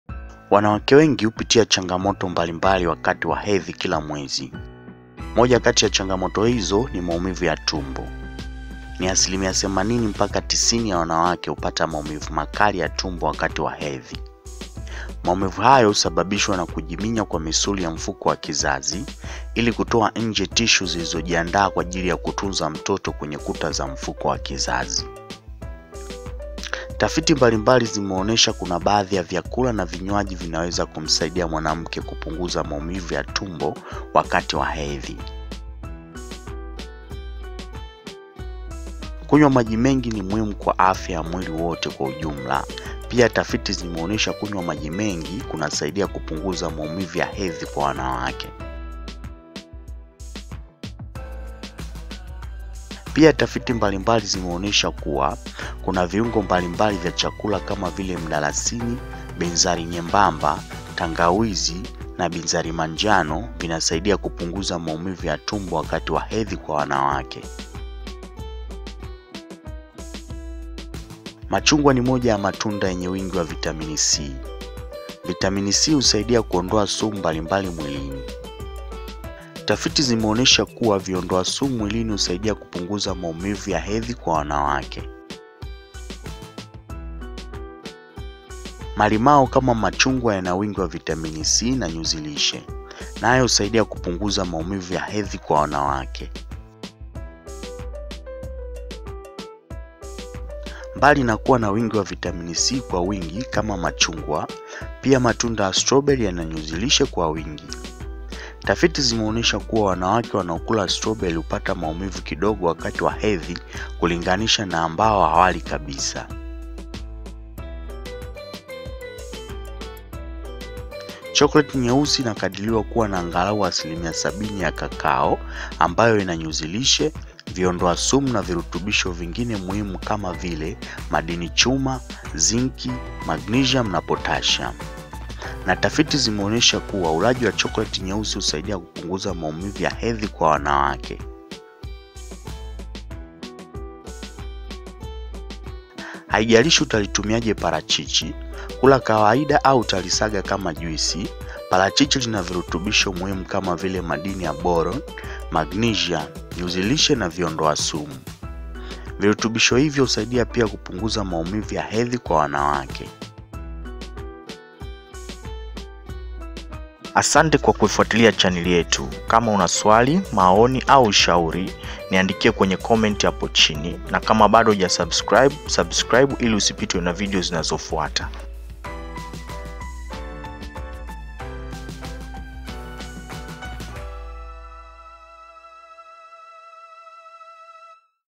Maumivu makali ya Tumbo wakati wa Hedhi | Tumia vitu hivi kupunguza maumivu
Katika video hii utafahamu kuhusu vitu mbalimbali ambavyo unaweza kuvitumia kupunguza maumivu kama unapata changamoto ya maumivu makali ya tumbo wakati wa hedhi.

Jiunge nasi katika safari hii ya kujifunza jinsi vitu vyetu vya asili vinavyoweza kuboresha maisha yako na kukufanya ujisikie bora kila siku.